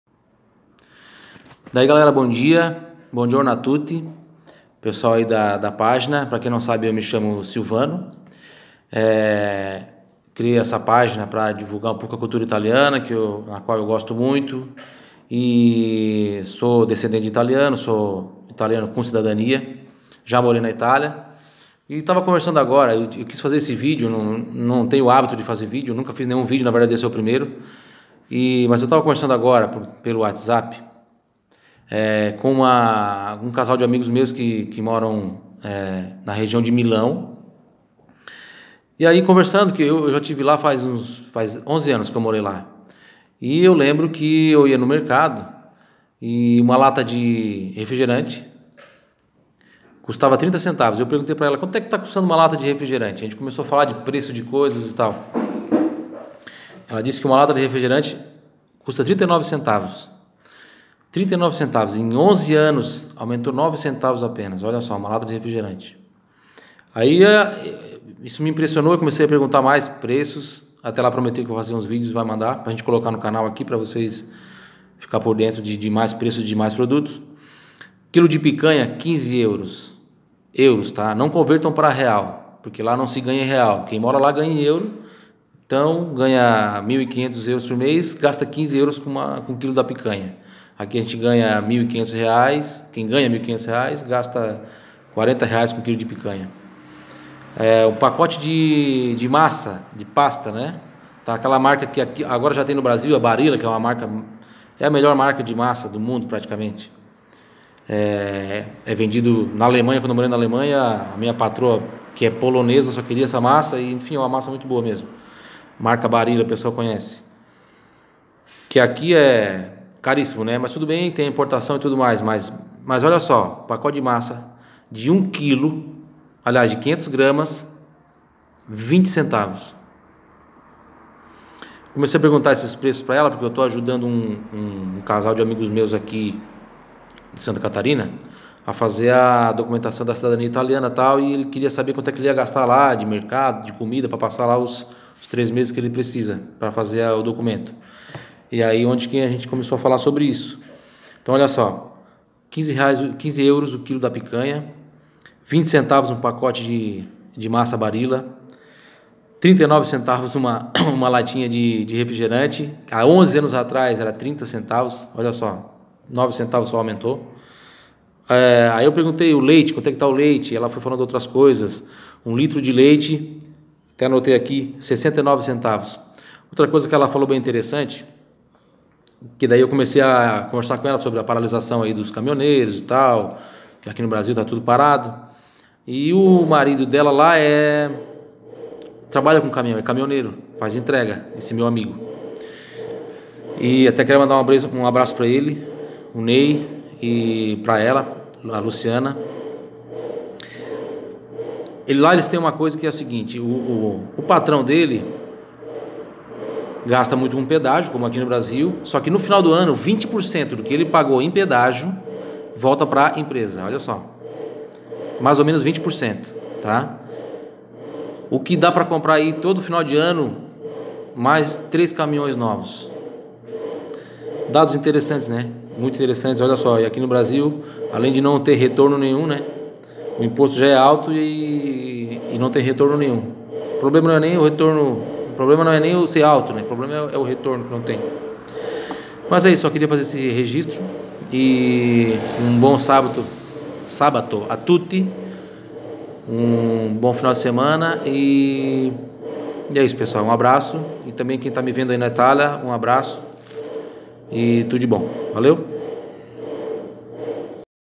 Subiu só 9 centavos em 11 anos.
Confira os preços de alguns produtos na Itália